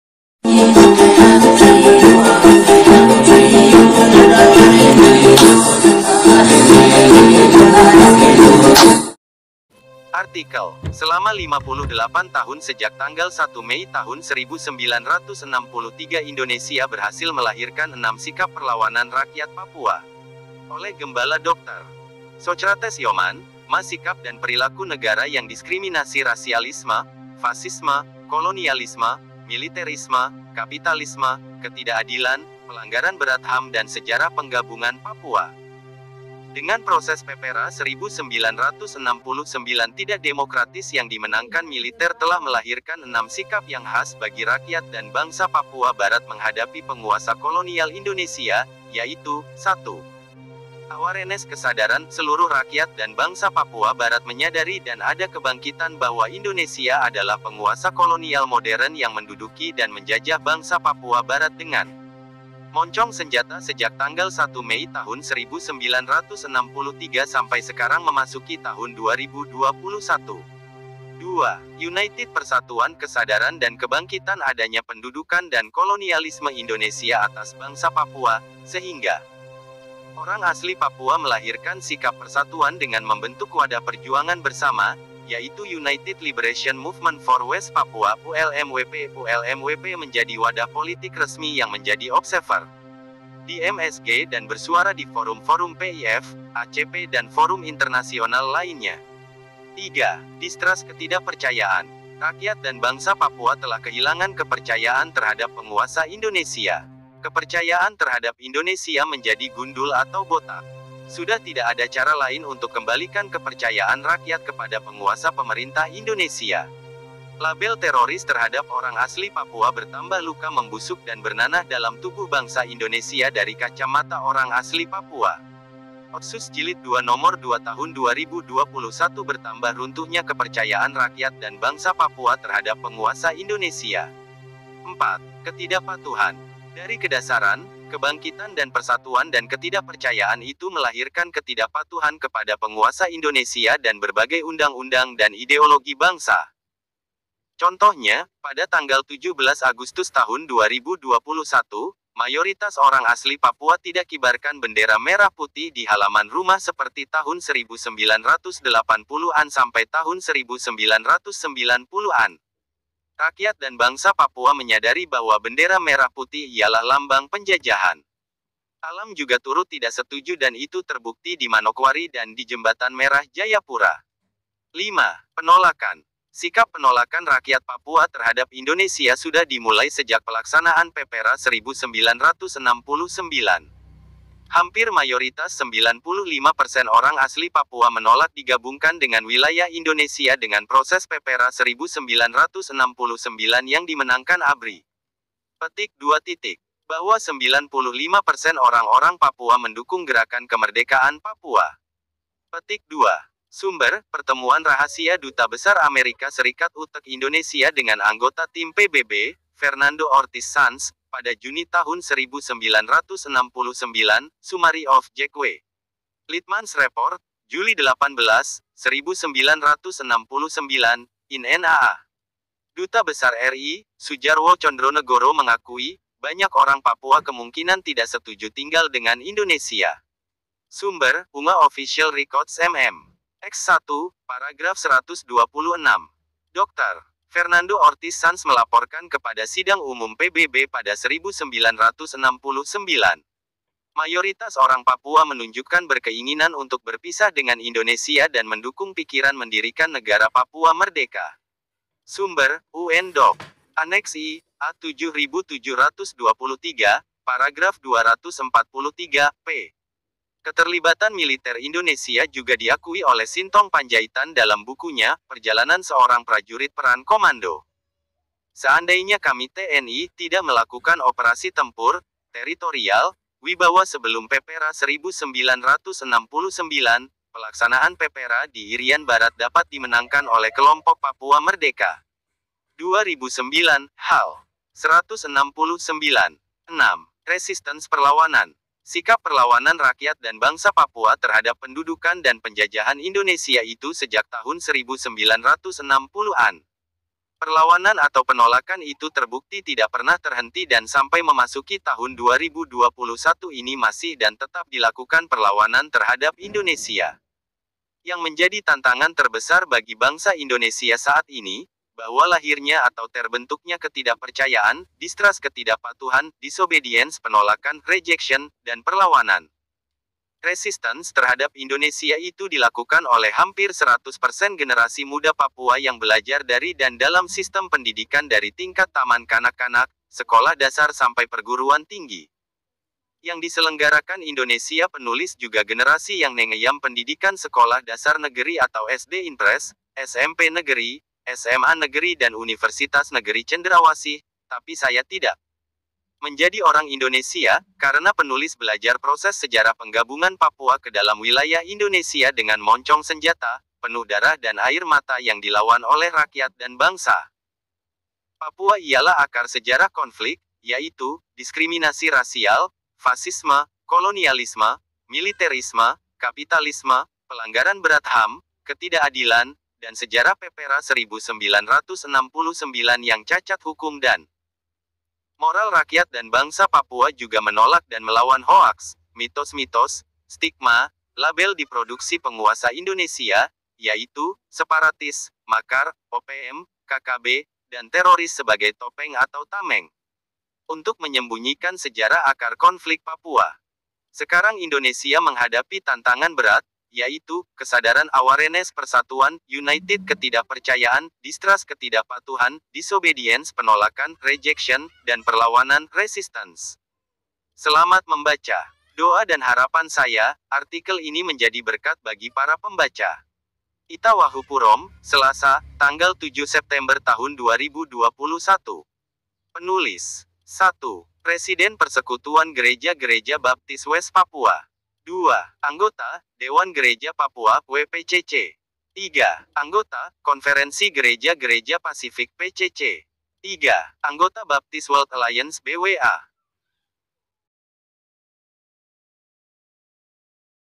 58 THN INDONESIA BERHASIL MELAHIRKAN 6 SIKAP PERLAWANAN RAKYAT PAPUA : Gem. Dr. Socratez Yoman,MA
SELAMA 58 TAHUN SEJAK 1 MEI 1963 INDONESIA BERHASIL MELAHIRKAN ENAM SIKAP PERLAWANAN RAKYAT PAPUA

Oleh Gembala Dr. Socratez Yoman,MA